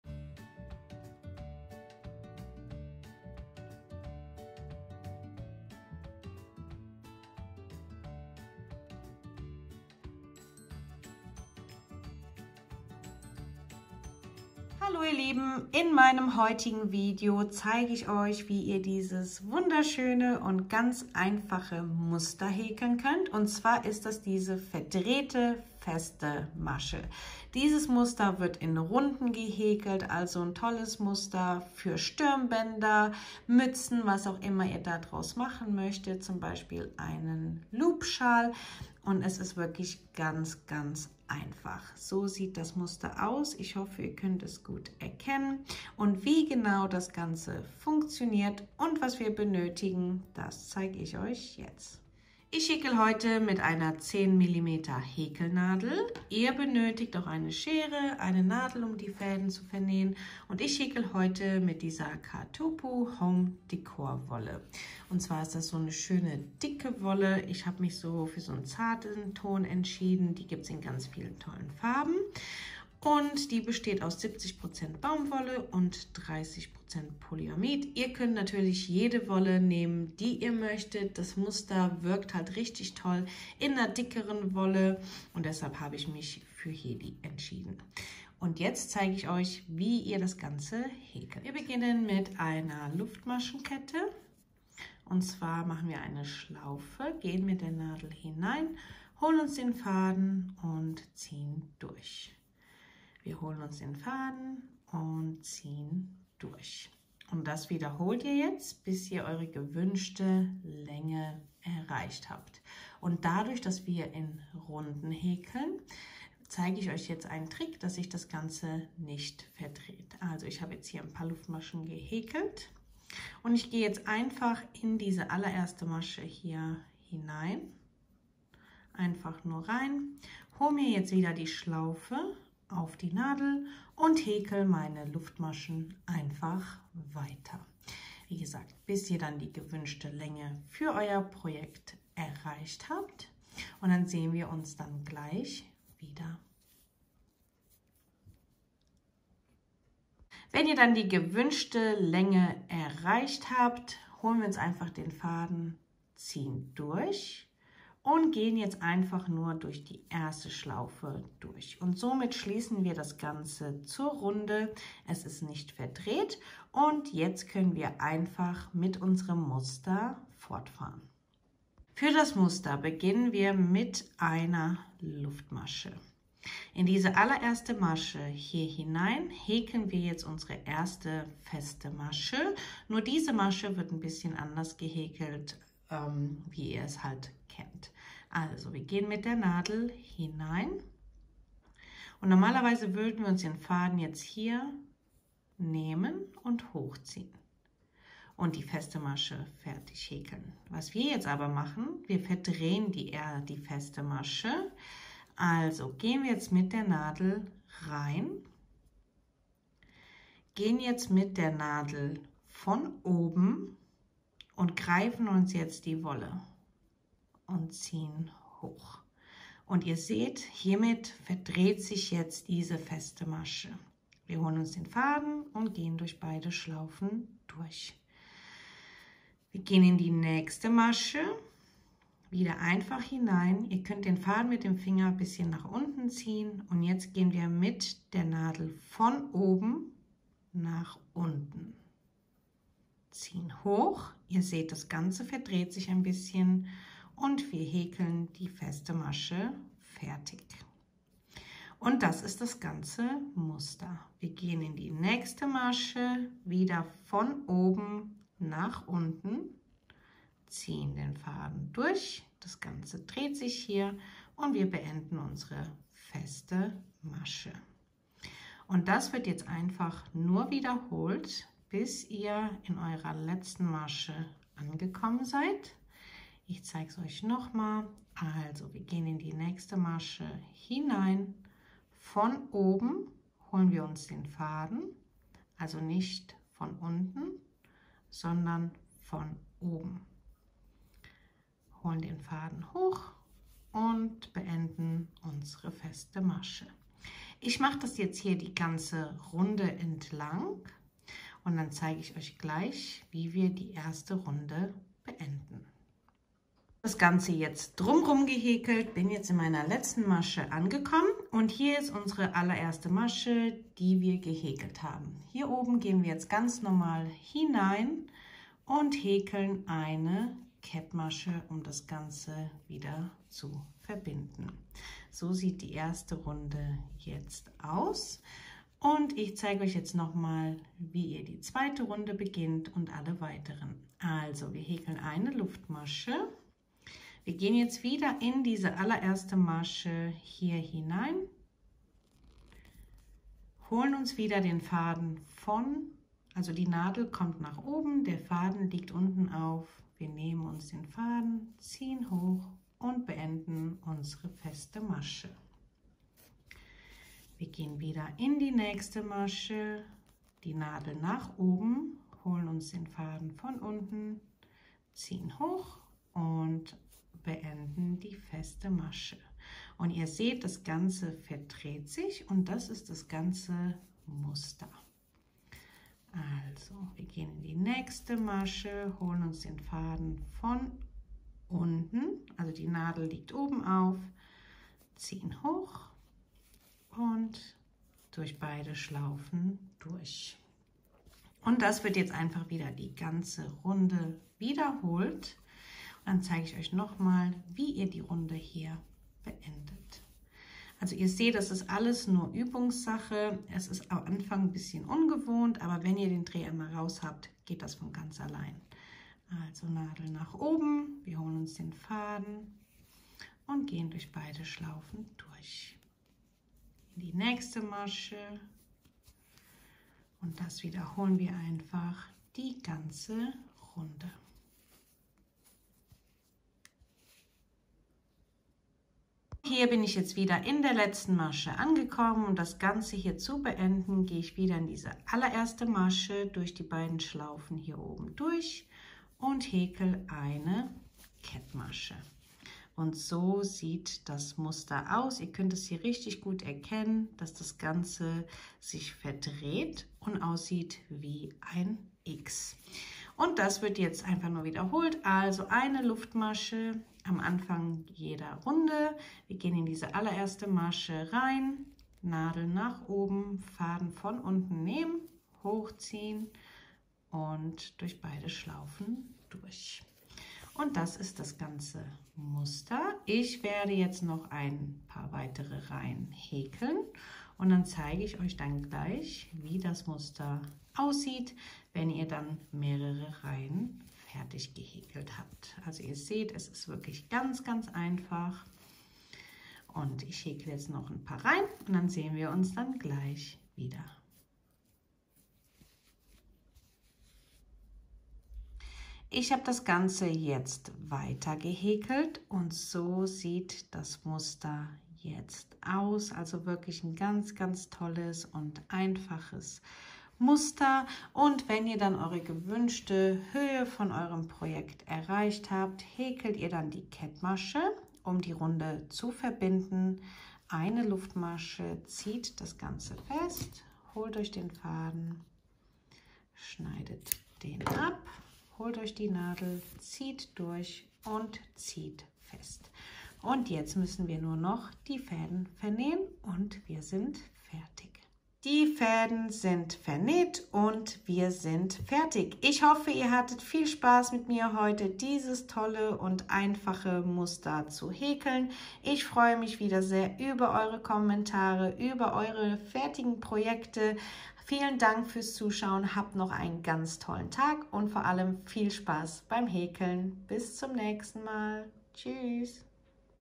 Verdrehte FESTE MASCHE häkeln
heute zeige ich euch wie ihr dieses tolle und einfache Muster häkeln könnt.
Die verdrehte feste Masche .
Ich wünsche euch ganz viel Spaß beim häkeln

*Benutzte Häkelnadel